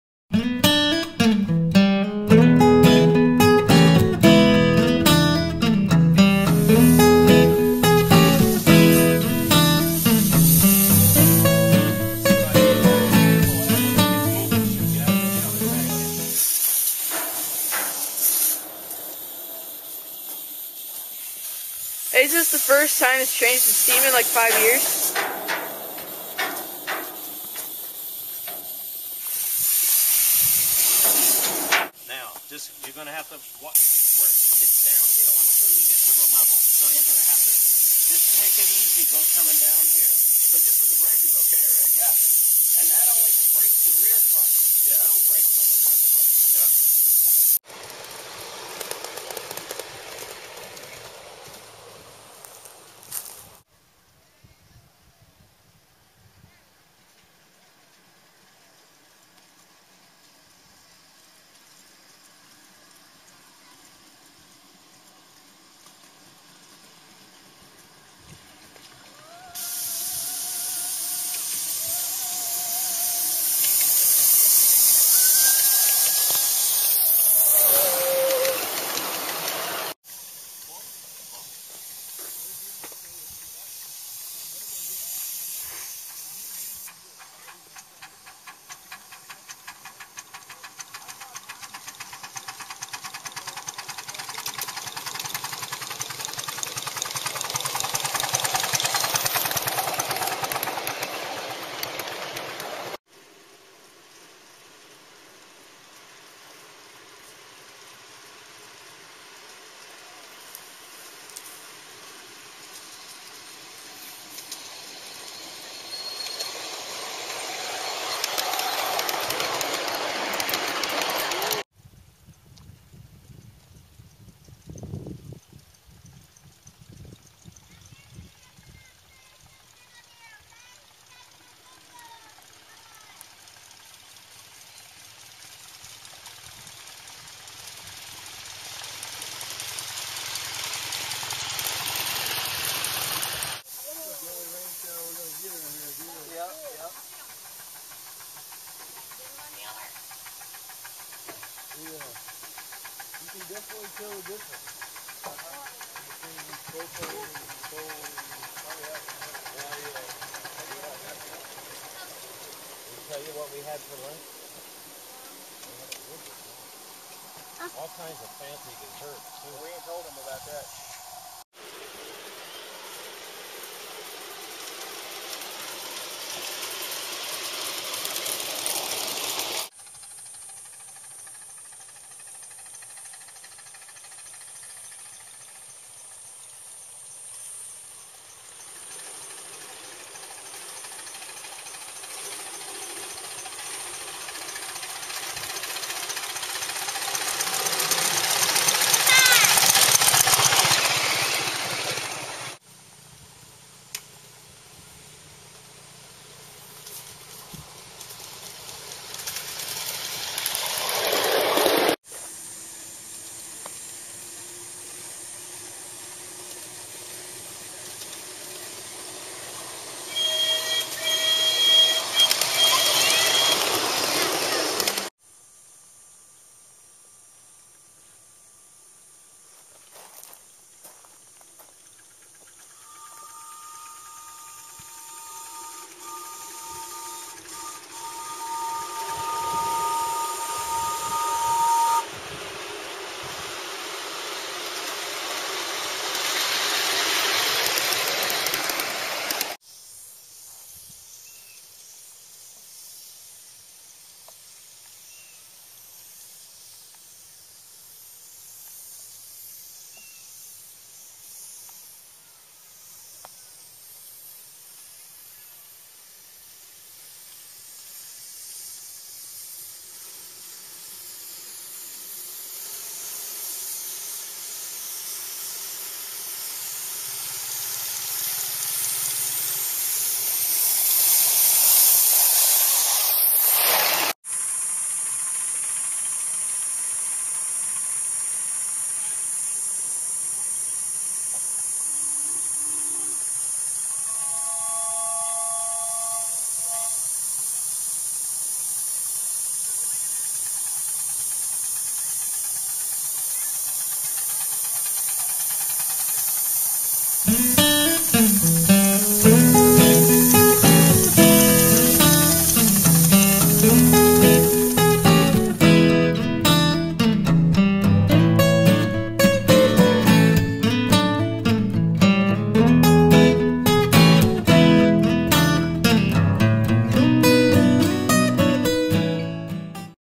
Mt. Nebo - Disney 4-4-0
Compilation of Mt. Nebo, a Disney 4-4-0 on the WB&S